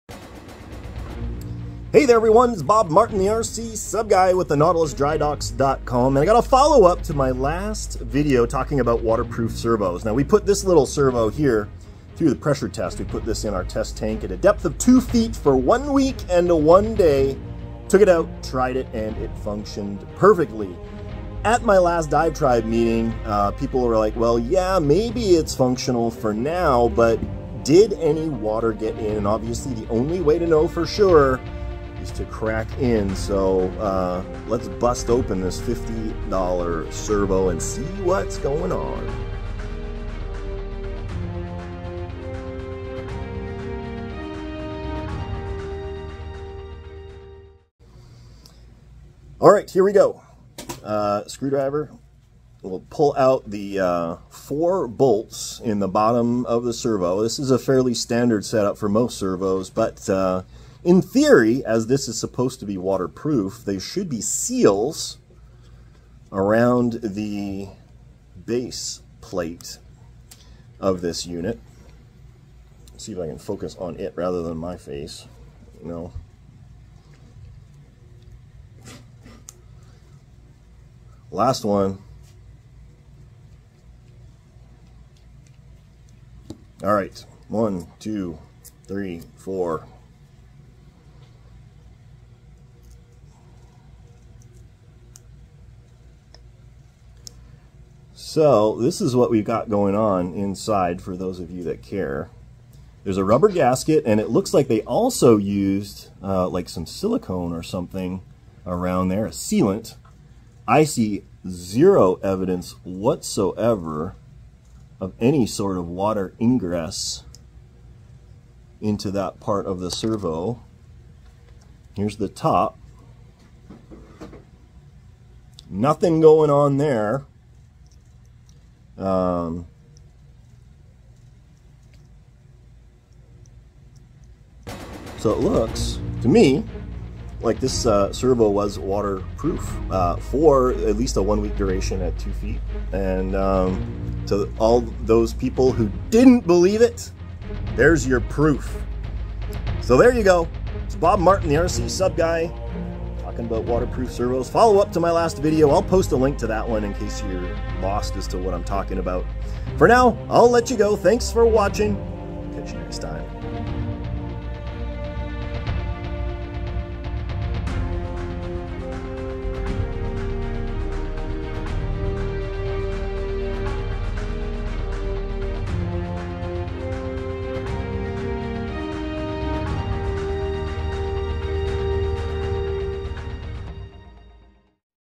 Waterproof servo testing UPDATE! What was inside after a week!?
By popular demand! We take apart the Hitec waterproof servo we dunked for a week at 2ft of depth and see what got into it.

The servo worked after a week, but did water actually get in!?

Just getting started and have questions?

Bob Martin
the RCSubGuy